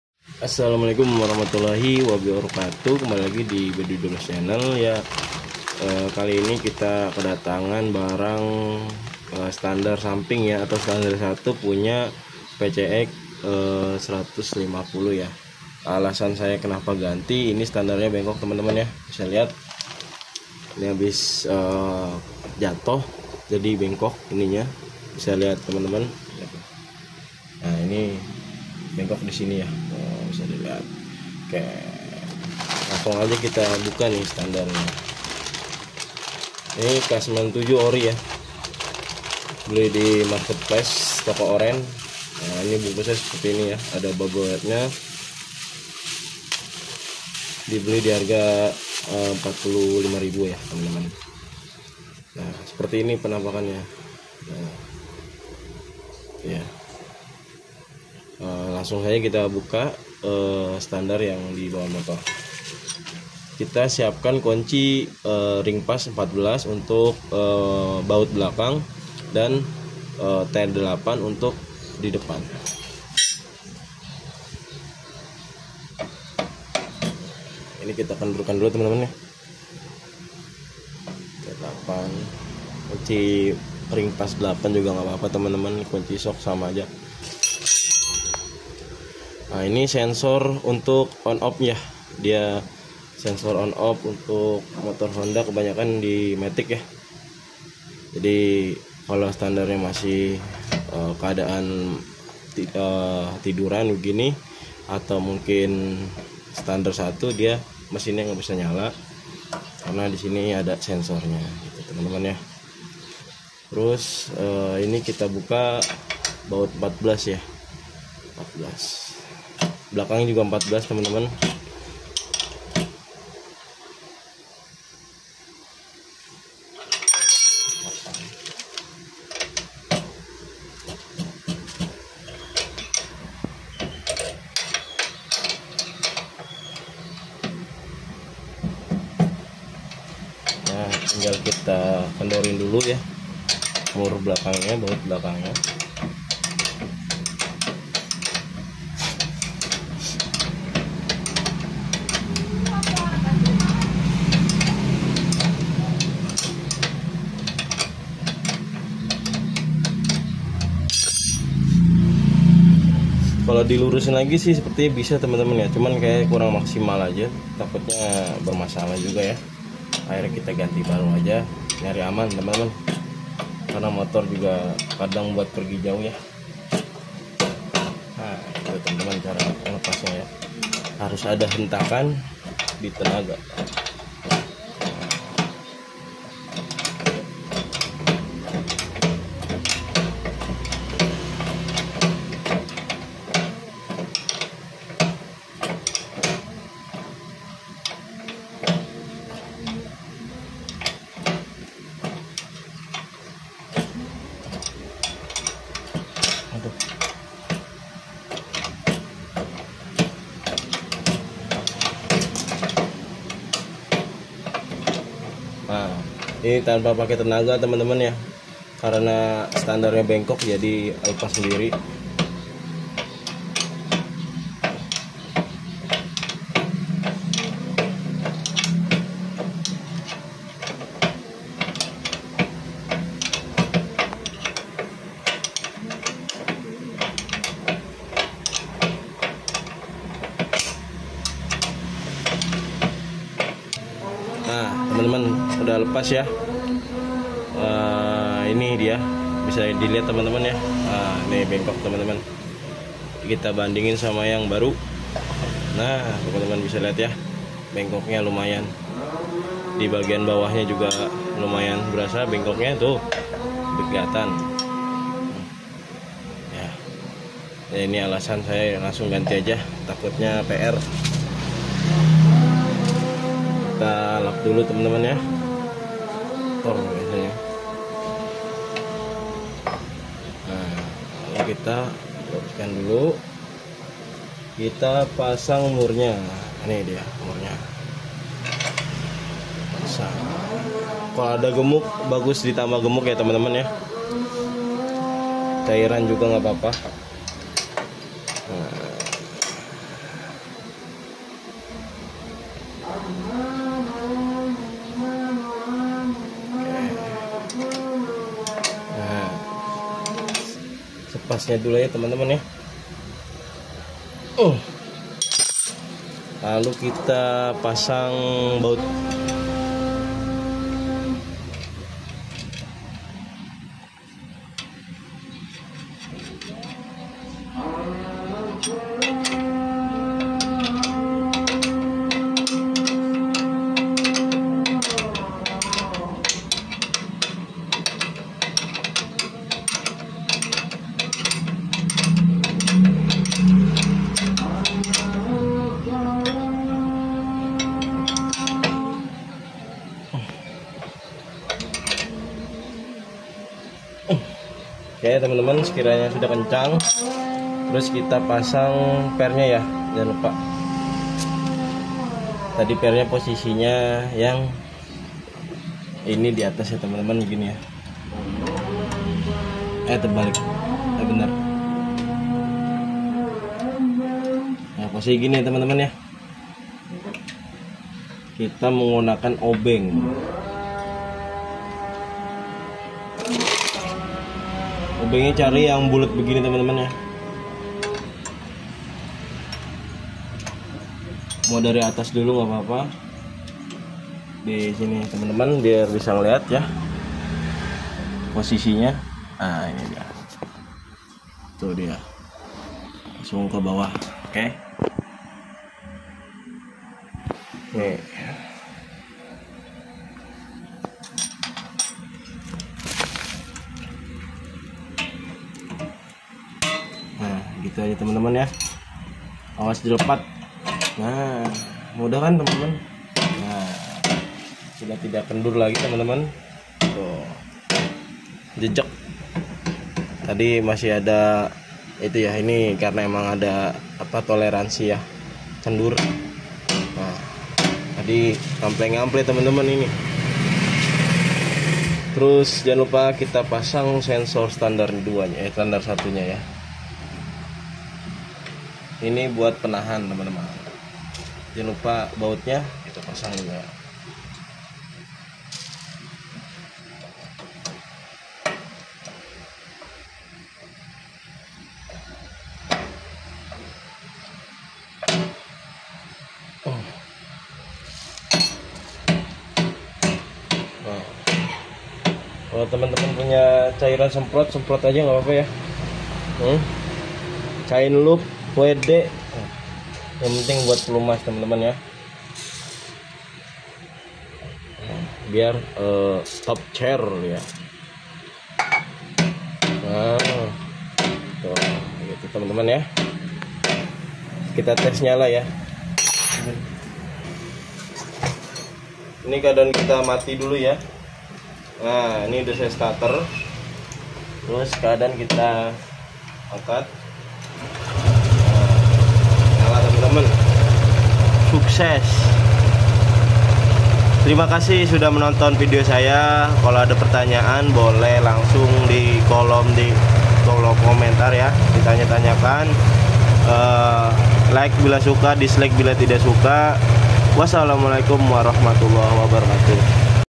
Menganti standar samping k97 pcx 150
yuk simak video tutorial cara Menganti standar samping pcx 150 berkode k97